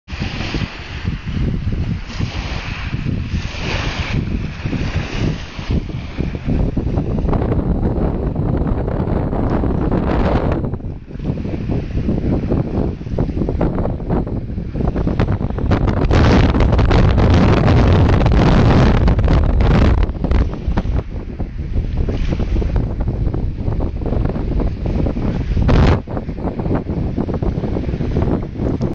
Snow on the beach
Stokes Bay and there's snow on the beach in April!